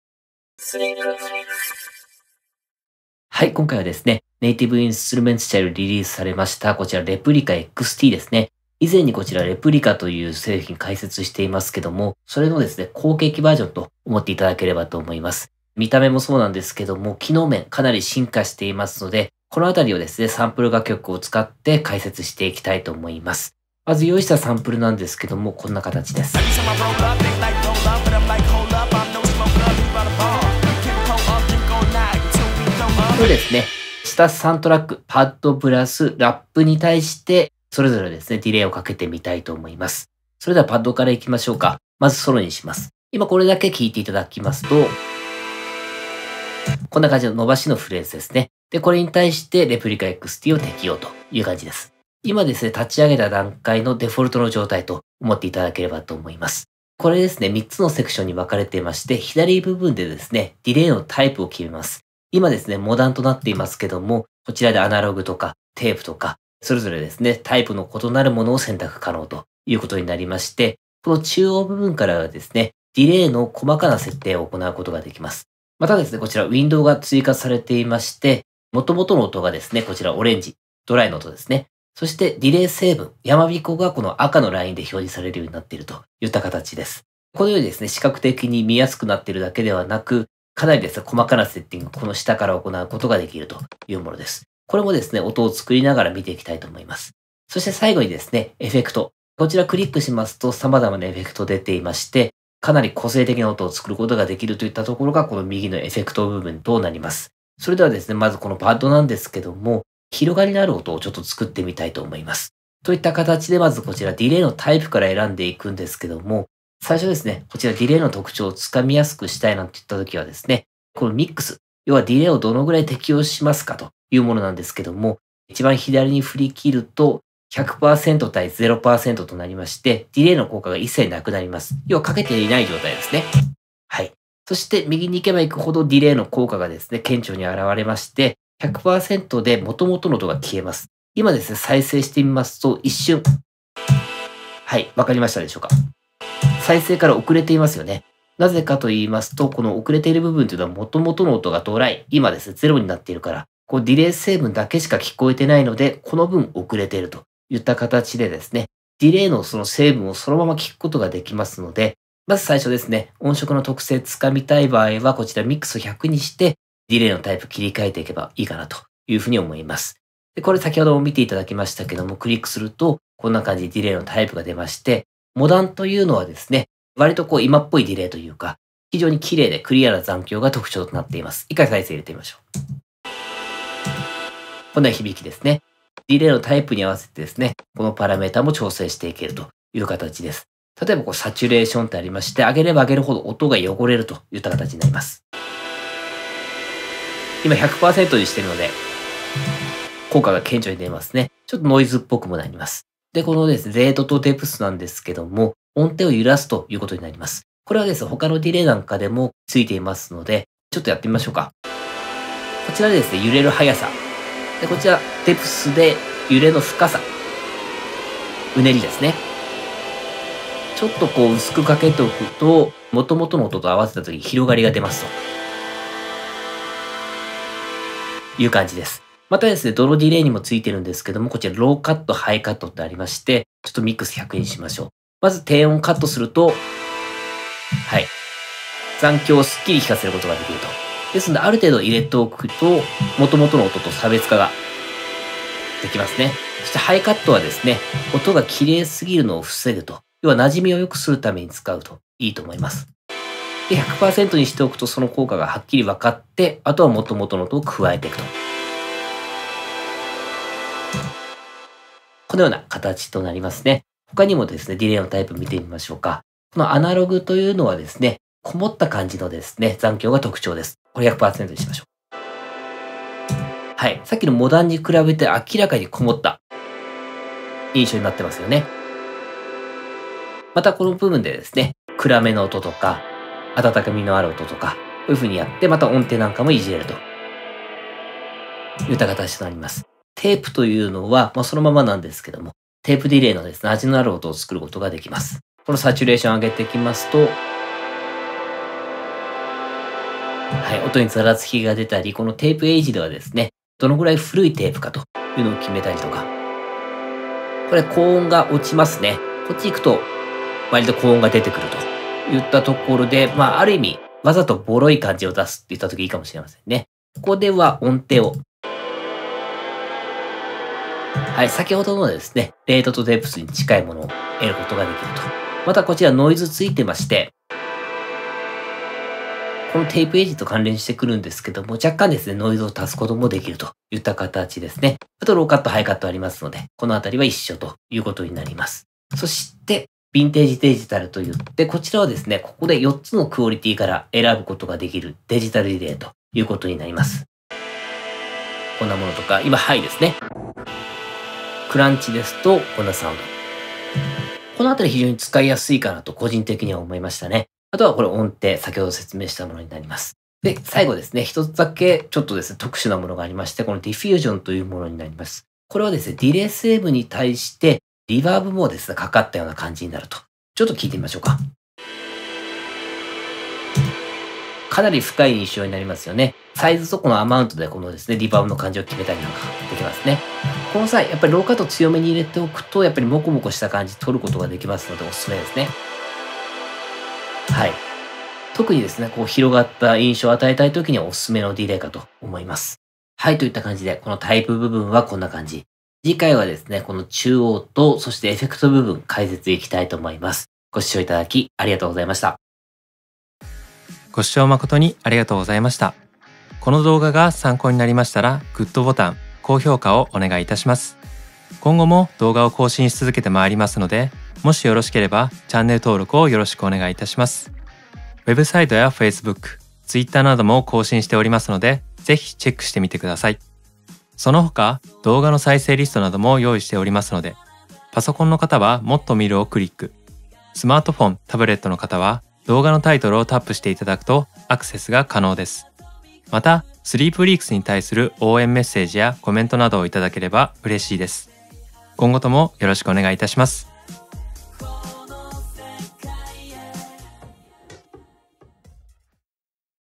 Native Instruments REPLIKA XTの使い方① 主要パラメーターの概要とコントロール（Sleepfreaks DTMスクール）
【解説 : スリープフリークス講師 金谷 樹】

Native Instruments社より「REPLIKA XT」がリリースされました。
以前に解説した「REPLIKA」の後継機で非常に多機能なディレイです。

前バージョンの出来も素晴らしいものでしたが、
「ディレイタイプの追加」「残響のグラフィカルな表示」「エフェクトの追加」など、
多くの進化を遂げています。

通常のディレイとしての役割はもちろん、
ディレイ成分を印象的に彩るエフェクティブな効果を演出することも可能です。

今回はREPLIKA XTの新機能に加えて、
主要パラメーターの概要とコントロール方法について詳しく解説していきます。